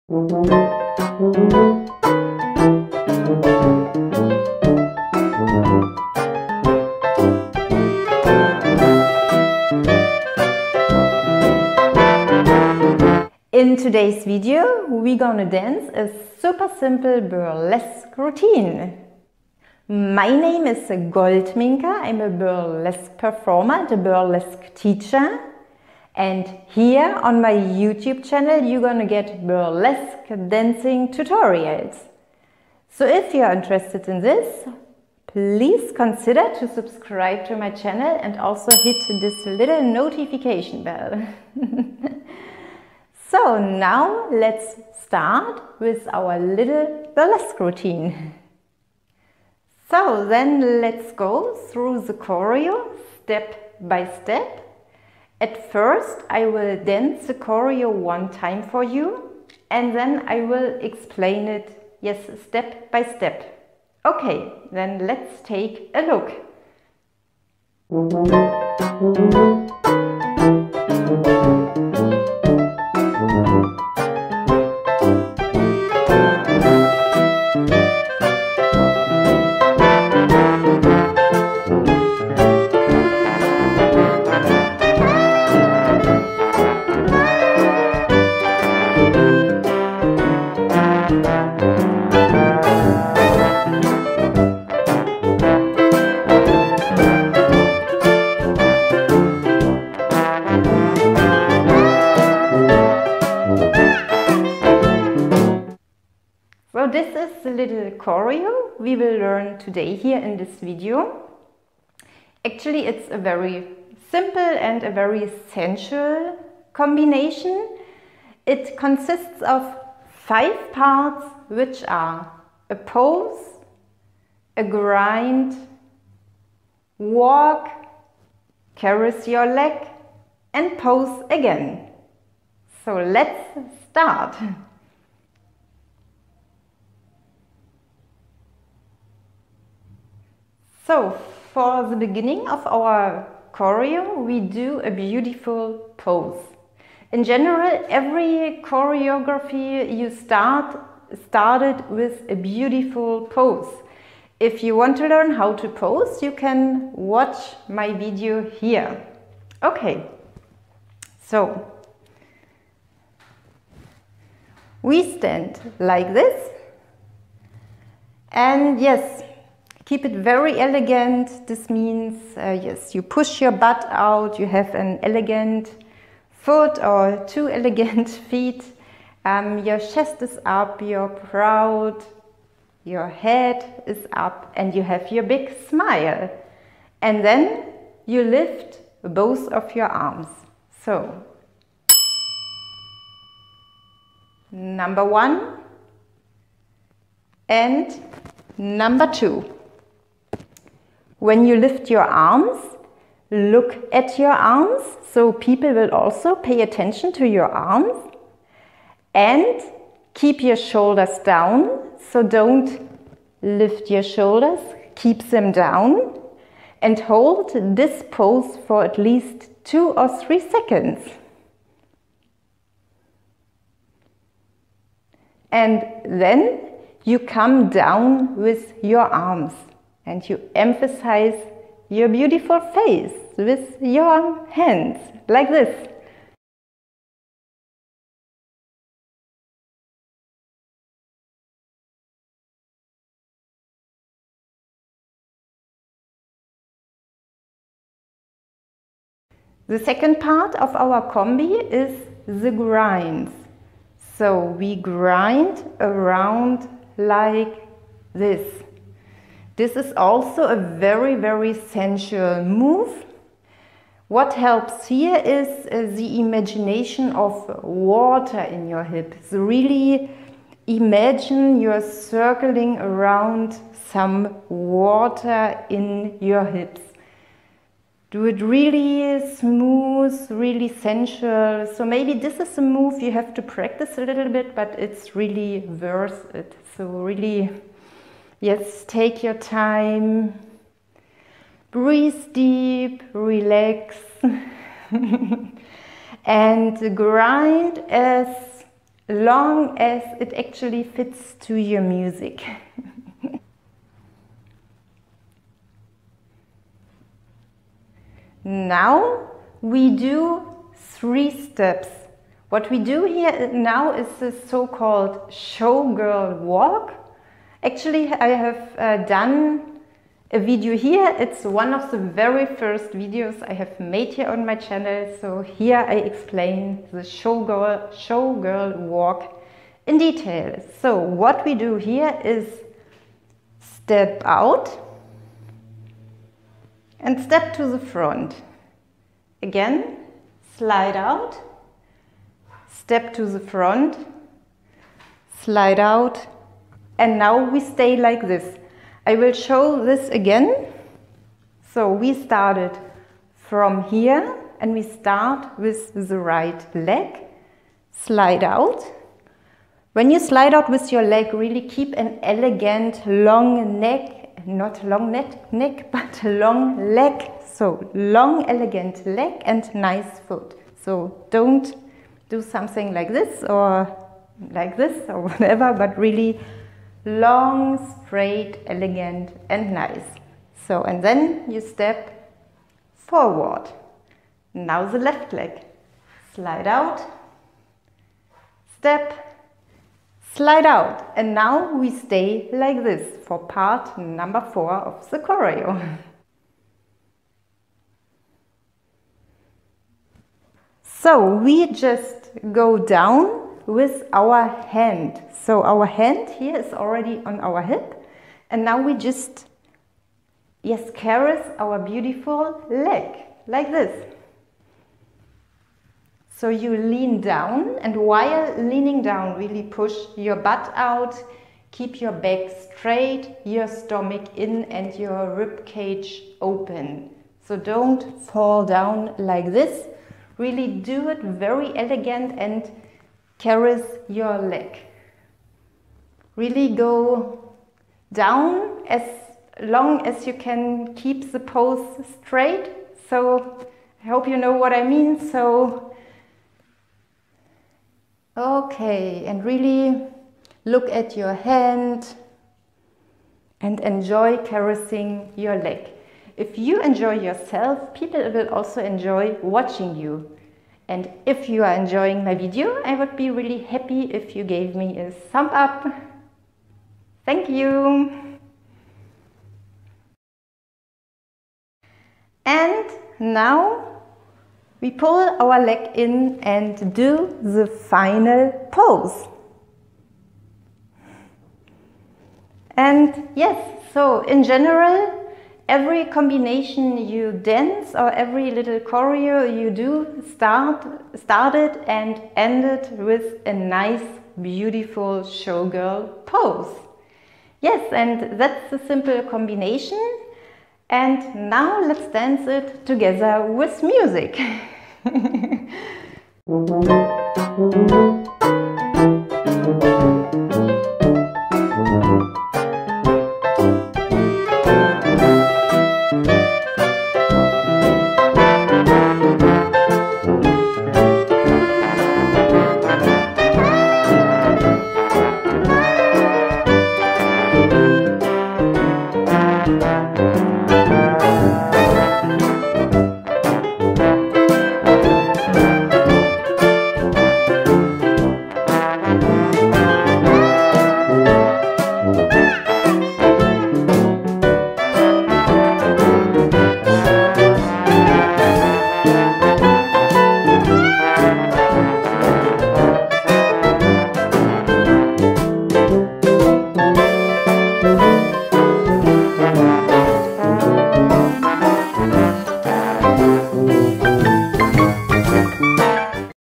Mini BURLESQUE DANCE Sequence for beginners - Burlesque Dance Tutorial
In this video you will learn a Mini Burlesque Dance Sequence in 5 steps:
1. Pose
2. Grind
3. Showgirl Walk
4. Caress your leg
5. Pose